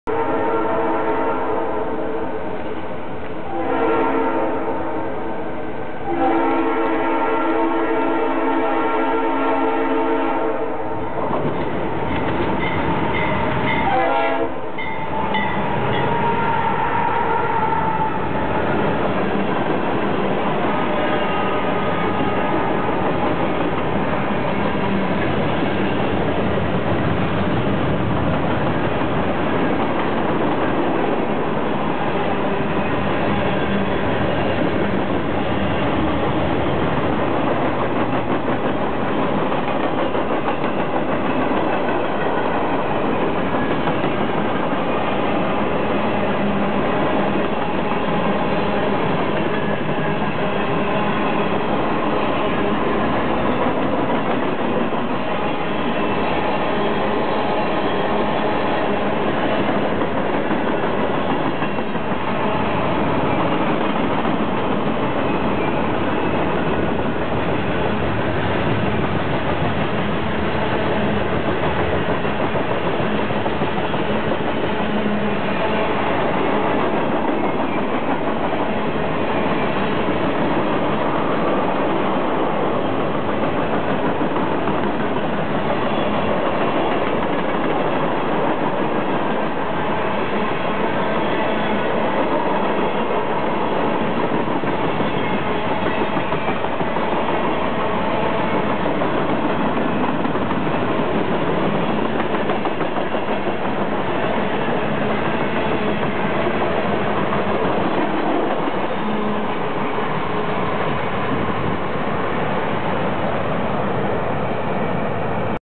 Trains in North Georgia -- NS Engines # 8362, 8035, and 7693
Norfolk Southern mixed freight train in Silver Creek, Georgia travelling north.
Three engines and 86 cars.